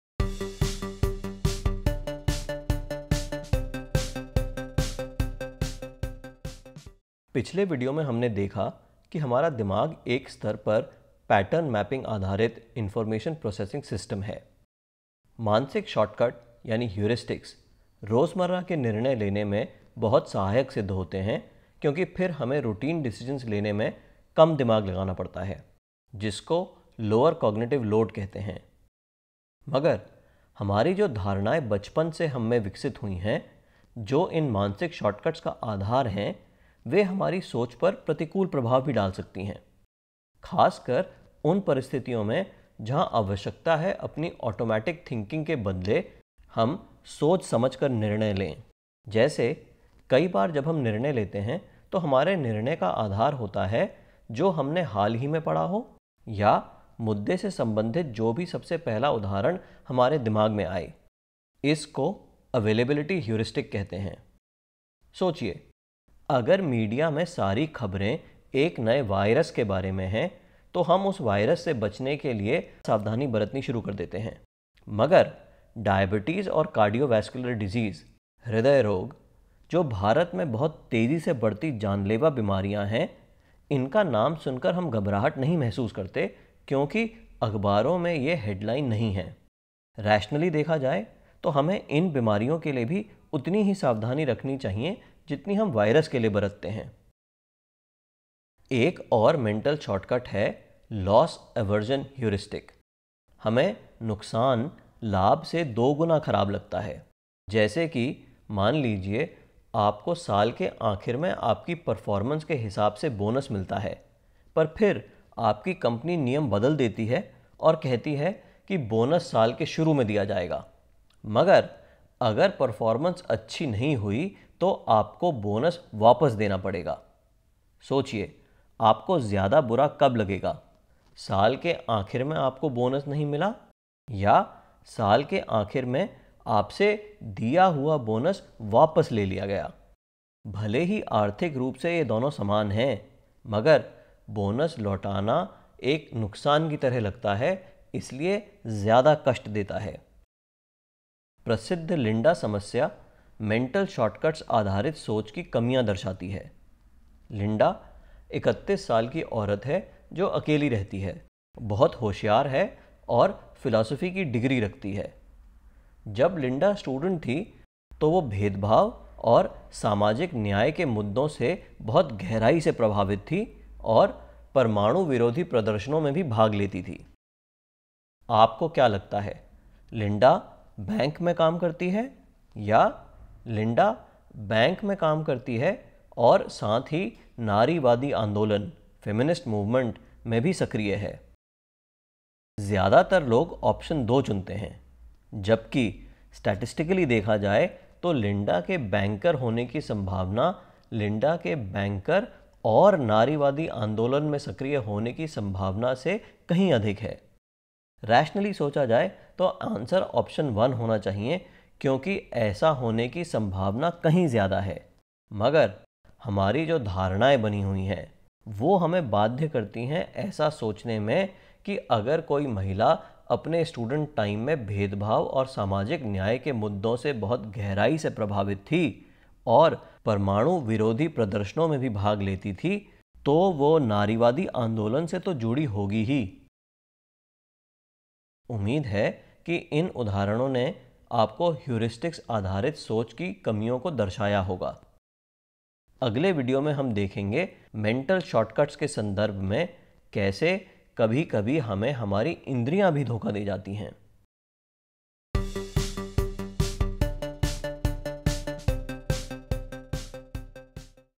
2. बाधक मानसिक शॉर्टकट्स
मानसिक शार्ट-कट यानि heuristics, रोज़मर्रा के निर्णय लेने में बहुत सहायक सिद्ध होते हैं क्योंकि फिर हमें routine decisions लेने में कम दिमाग लगाना पड़ता है। मगर, हमारी जो धारणाएँ बचपन से हममें विकसित हुईं हैं, जो इन मानसिक शॉर्टकट्स का आधार हैं, वे हमारी सोच पर प्रतिकूल प्रभाव भी डाल सकती हैं, खासकर उन परिस्थितियों में जहाँ आवश्यकता है अपनी आटोमेटिक थिंकिंग के बदले हम सोच-समझ कर निर्णय लें। इस वीडियो में हम समझते हैं की कैसे कभी-कभी मानसिक शॉर्टकट्स यानि heuristics-based thinking बाधक साबित होती है।